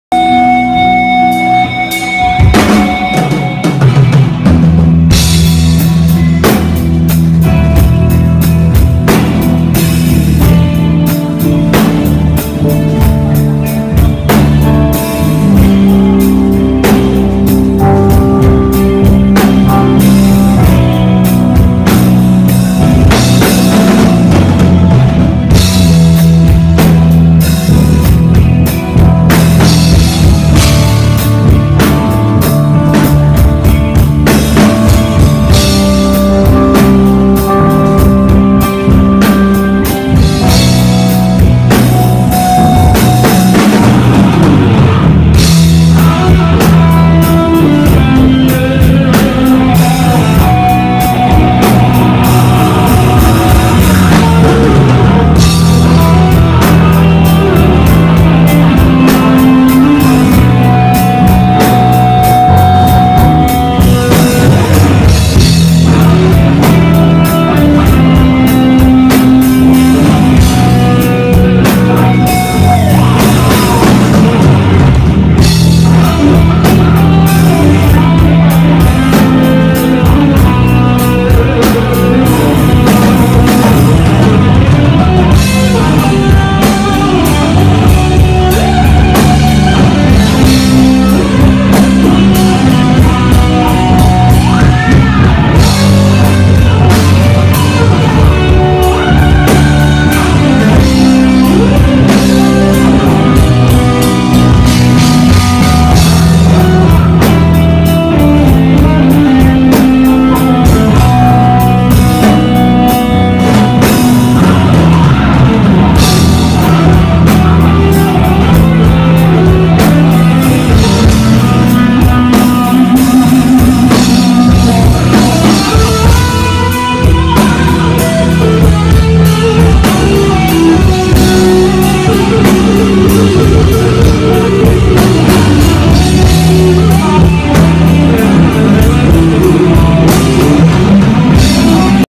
KARNAFULLY LIGHTING..Guitar Instrumental Solo Of AB, Lighting Sponsor by Karnafully Lighting..
KARNAFULLY LIGHTING..Guitar Instrumental Solo Of AB, Lighting Sponsor by Karnafully Lighting at Radisson Blu Ctg..